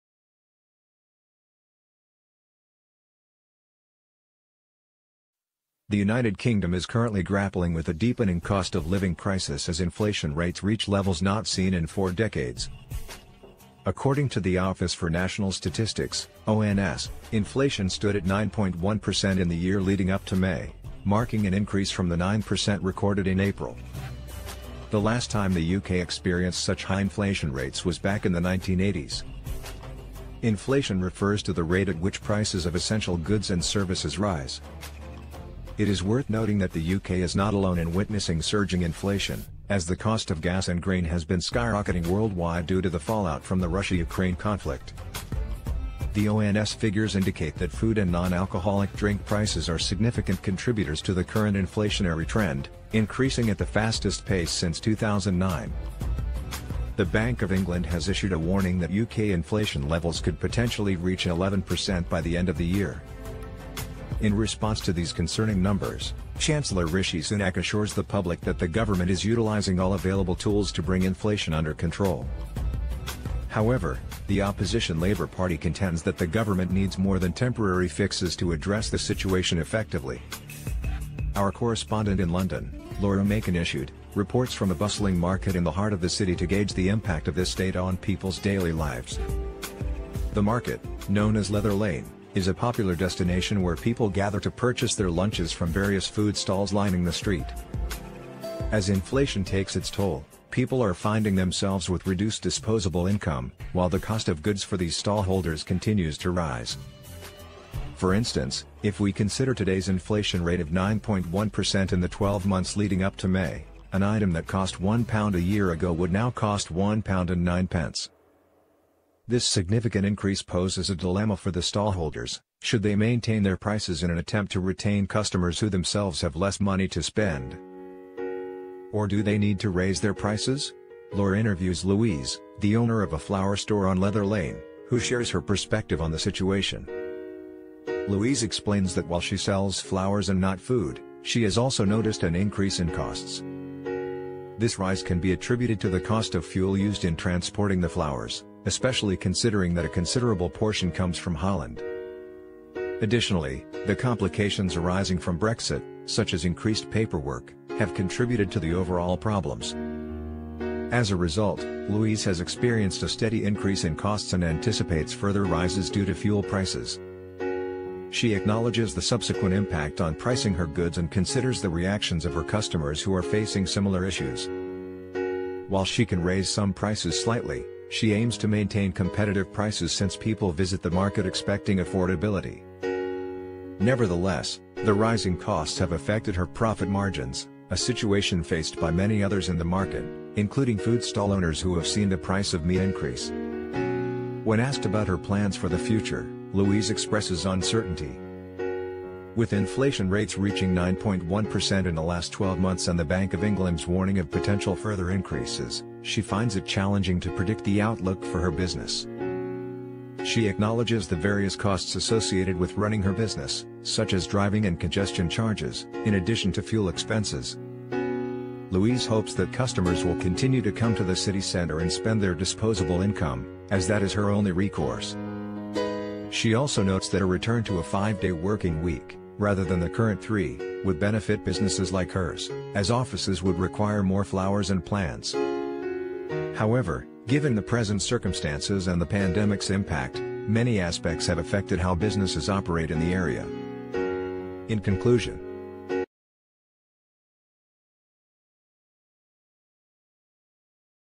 UK Faces Intensifying Cost of Living Crisis as Inflation Soars: Real-Life Implications Revealed
Inflation in the UK reaches a four-decade high, causing a severe cost of living crisis. This article explores the real-life impact of soaring prices on traders and consumers in a bustling London market.